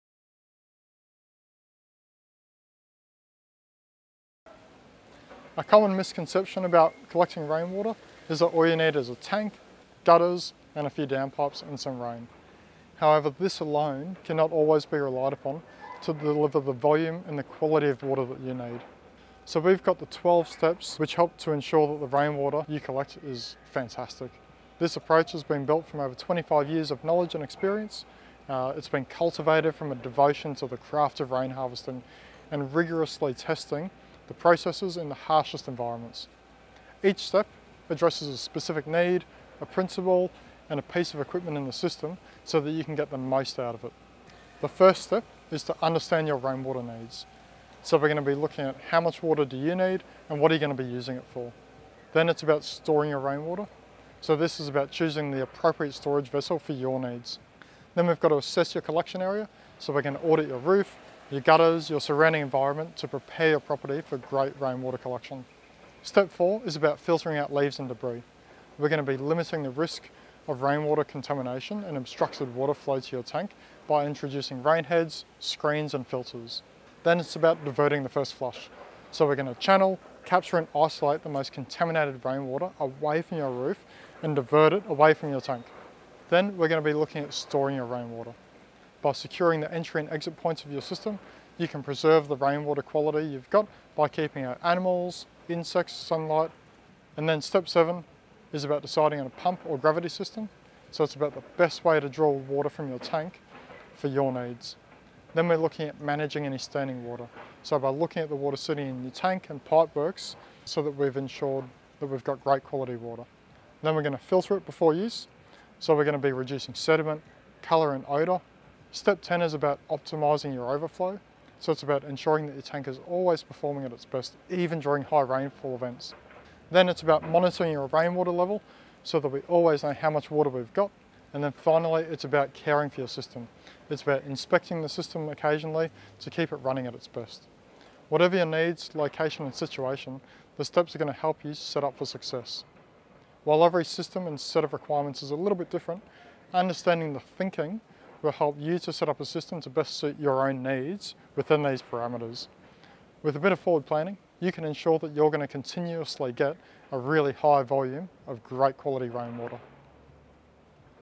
12 Steps of Rain Harvesting Overview
A common misconception about collecting rainwater is that all you need is a tank, gutters, a few downpipes, and some rain.
However, this alone cannot be relied upon to deliver the volume and quality of water you need.

Instead, follow the 12 Steps to ensure your rainwater harvesting success. This approach has been built from over 25 years of knowledge and experience, cultivated from a devotion to the craft of rain harvesting and rigorously testing the process in the harshest environments.